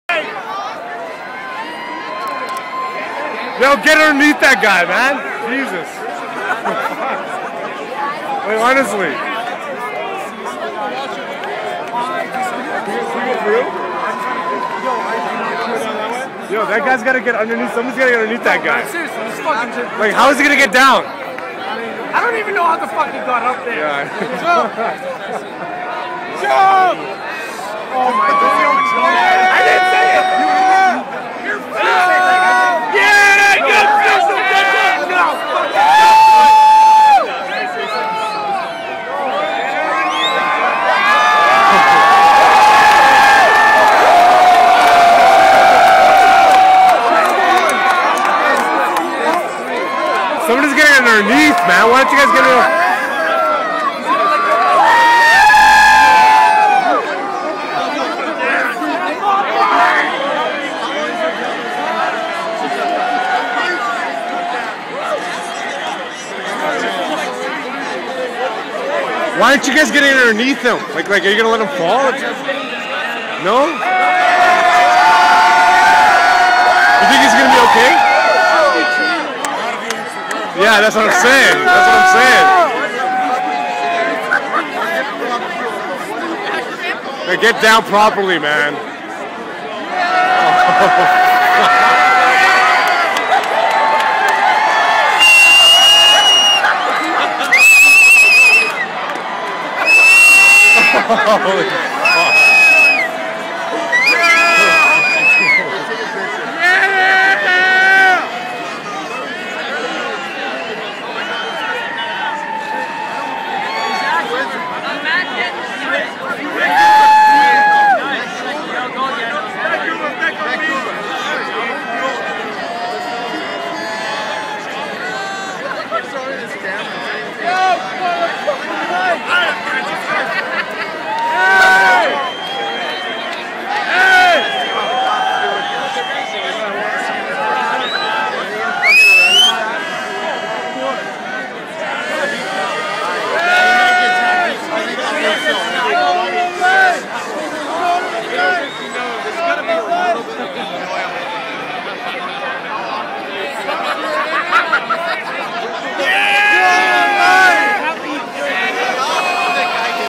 Drunk guy on traffic light
vancouver 2010 olympics guy drunk on traffic light